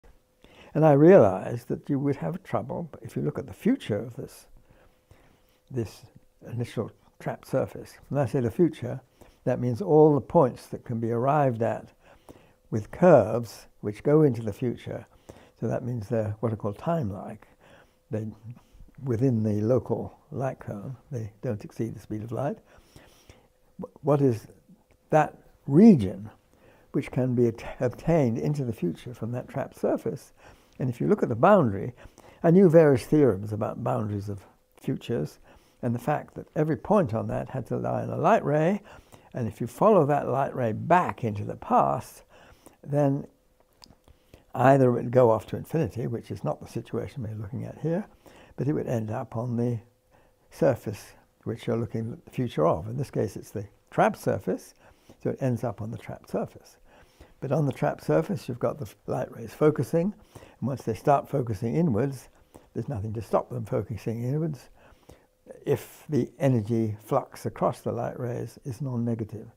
Exploring the Future of Trapped Surfaces in Physics
And I realized that you would have trouble if you look at the future of this this initial trapped surface When I say the future that means all the points that can be arrived at with curves which go into the future So that means they're what are called time-like Within the local light cone they don't exceed the speed of light What is that region which can be obtained into the future from that trapped surface And if you look at the boundary I knew various theorems about boundaries of futures and the fact that every point on that had to lie in a light ray And if you follow that light ray back into the past then Either it would go off to infinity which is not the situation we're looking at here but it would end up on the surface which you're looking at the future of In this case it's the trapped surface so it ends up on the trapped surface But on the trapped surface you've got the light rays focusing and once they start focusing inwards there's nothing to stop them focusing inwards if the energy flux across the light rays is non-negative @
If you enjoy our content and want to help us, consider making a donation! Every little bit helps us improve and bring you more of what you love.